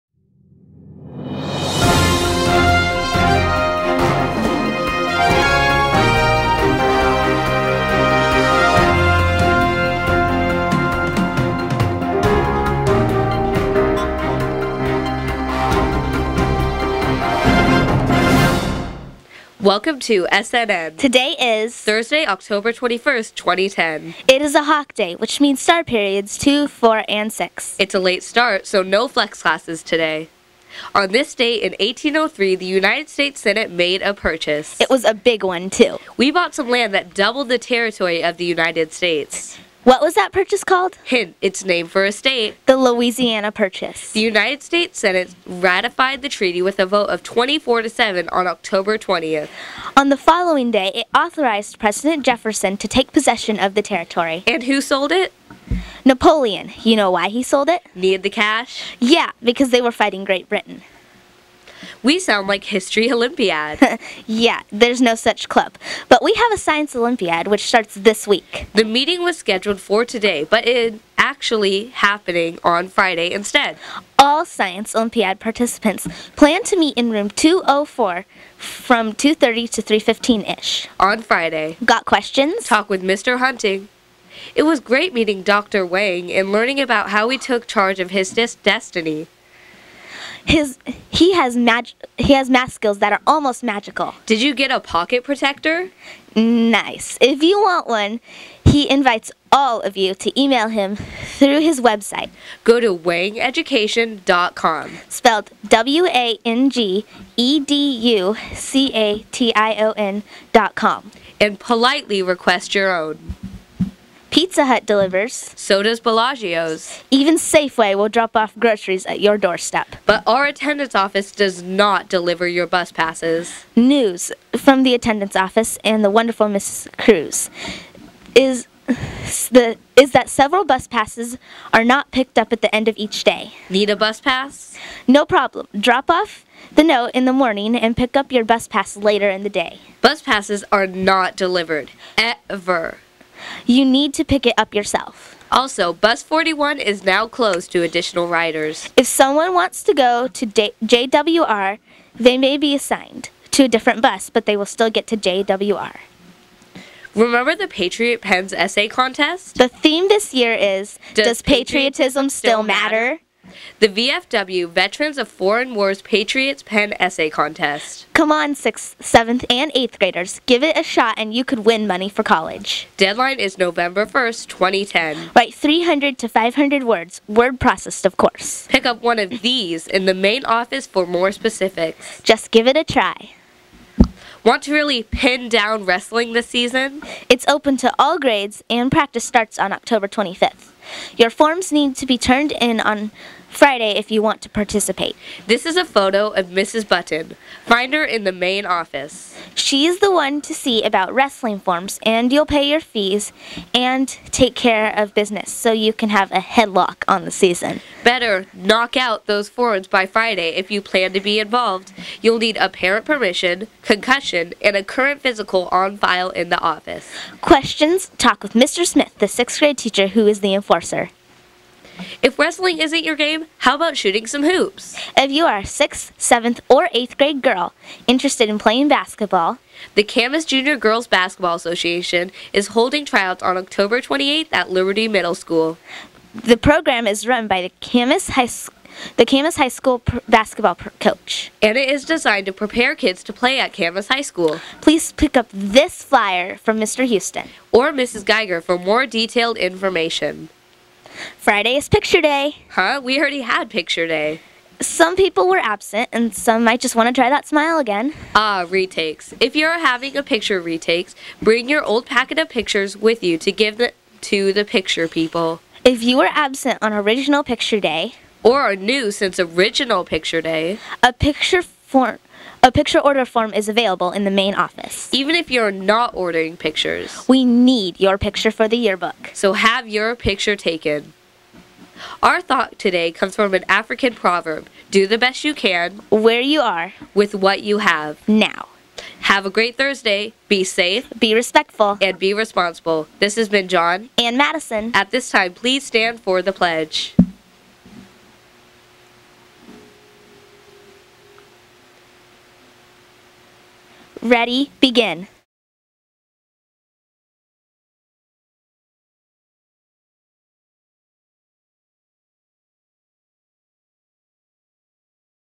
Skyridge Network News LIVE 10.21.10
Jon and Madison share news about the Louisiana Purchase, Science Olympiad meeting on Friday, basketball, wrestling and the Patriot's Pen Essay contest. Also, student need to remember to pick up their bus passes at the attendance office.